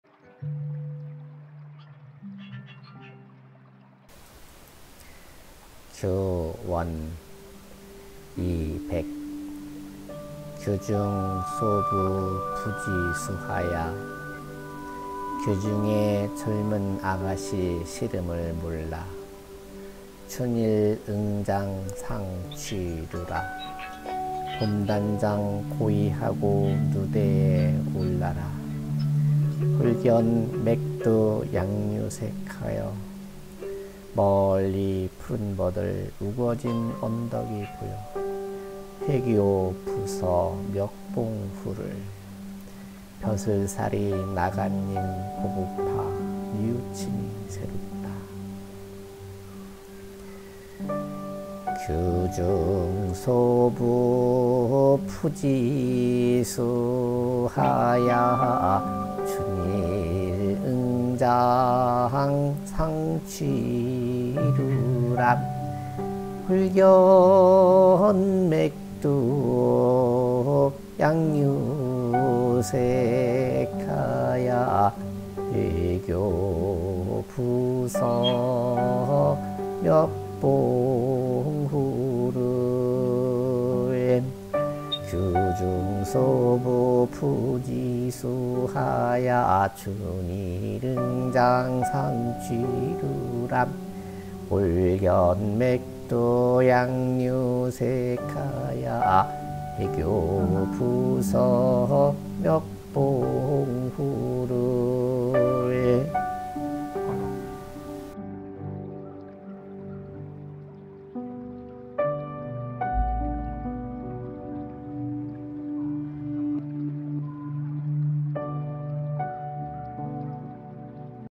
한시 강독 : 閨怨
봄이 오니,
사람들 꽃 구경하여
덩달아 높은 누각에 올라보네.
쌍쌍이 손잡고 봄 놀이 하는데,
나 홀로 님은 먼곳에
출세길 찾아 떠나버렸으니
아이 이을 어쩌나!